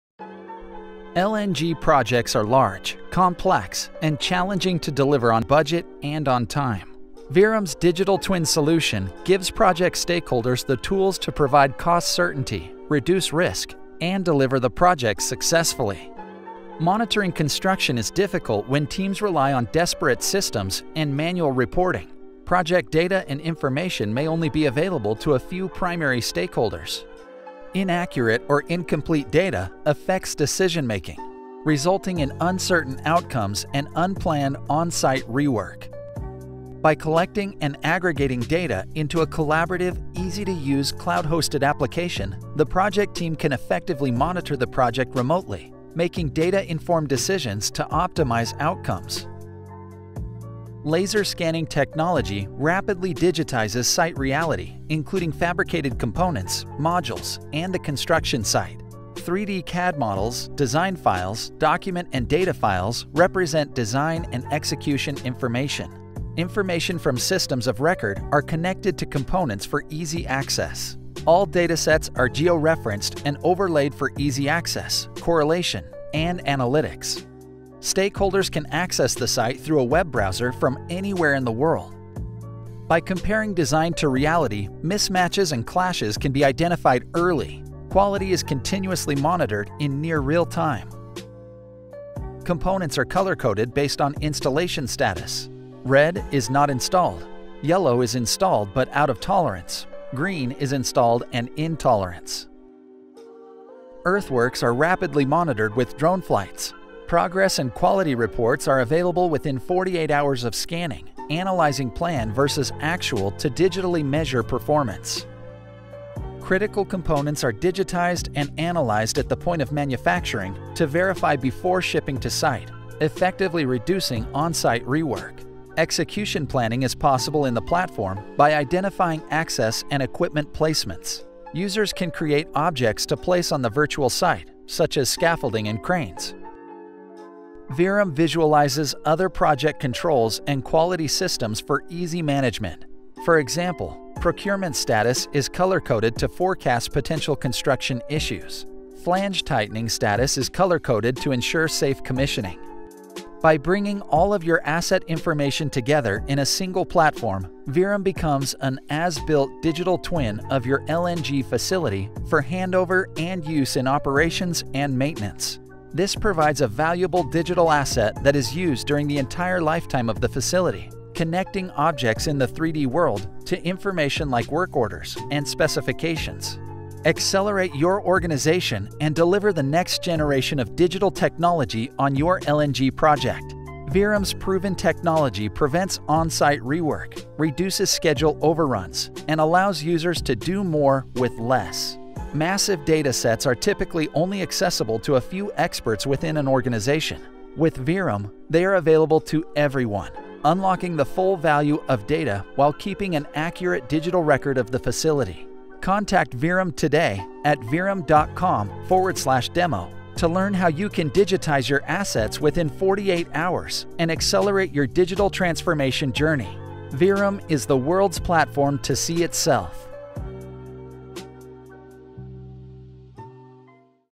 VEERUM DigitalTWIN for construction progress visualization | VEERUM solution demonstration
Accelerate your organization's digital transformation strategy and deliver the next generation of digital twins on your LNG project with VEERUM. Our proven solutions prevents on-site rework, reduces schedule overruns, and allows users to do more with less.

to get a live demo and learn how you can digitize your assets within 48 hours.